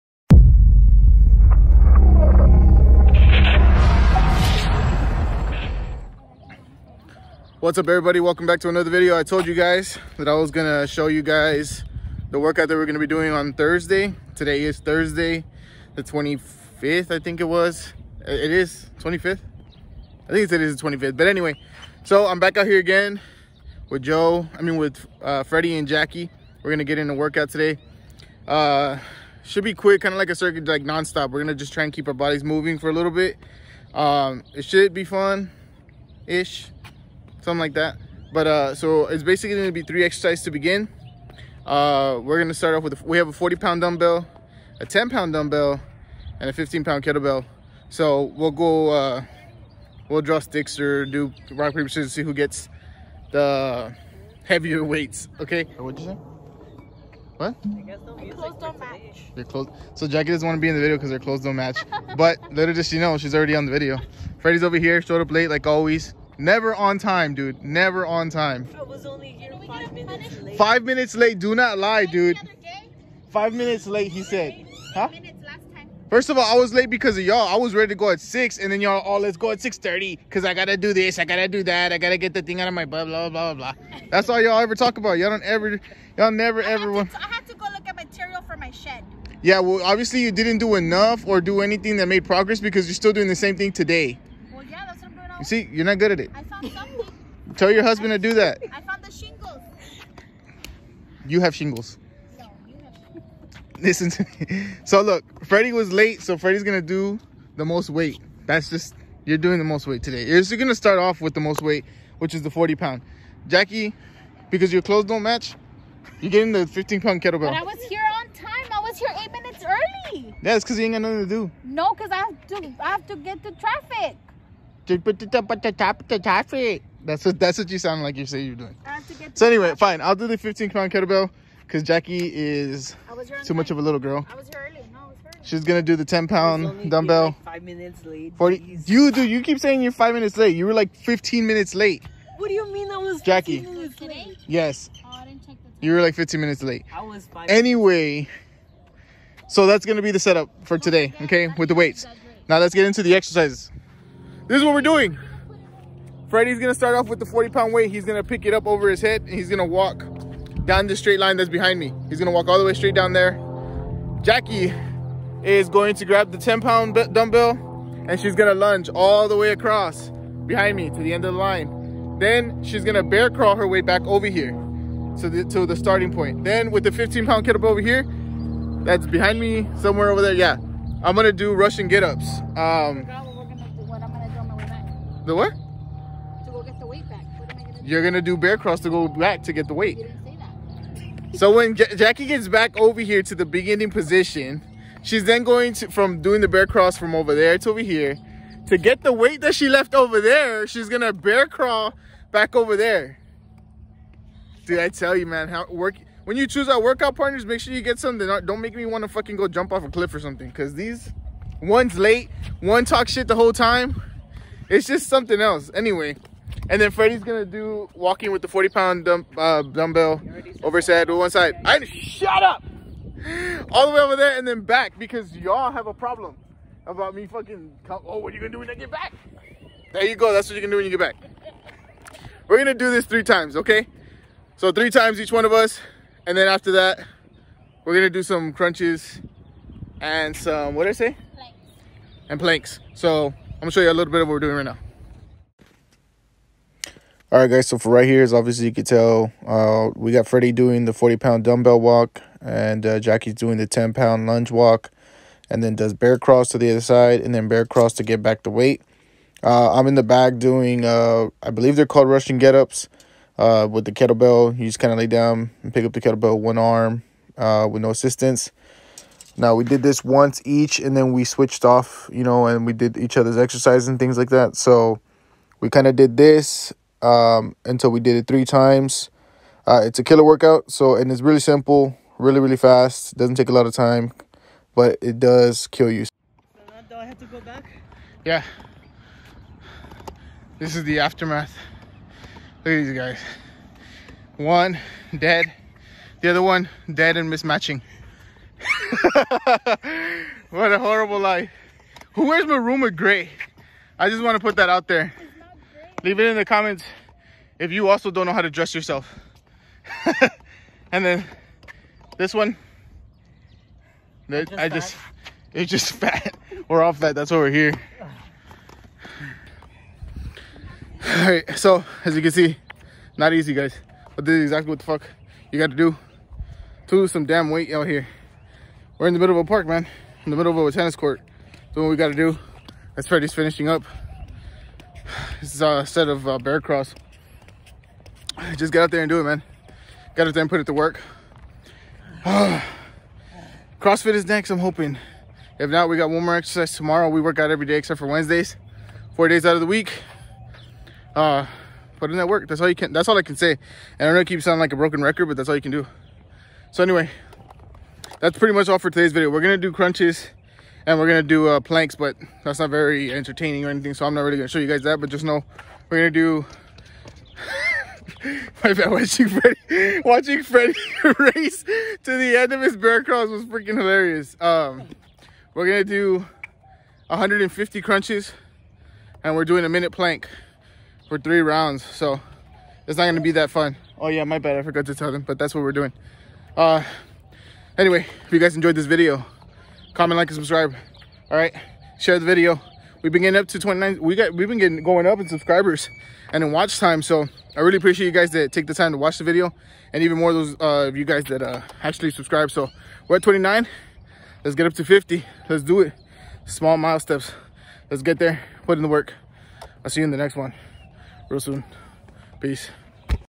Fast Workout Circut to lose weight (PAINFUL)
Unleash Your Inner Wolf!

So today I wanted to show you guys the painful circut that we did. It seems simple but after doing it myself, I can confirm that it's not as easy as it seems. Try it out for yourself and let me know what you think. You can do it!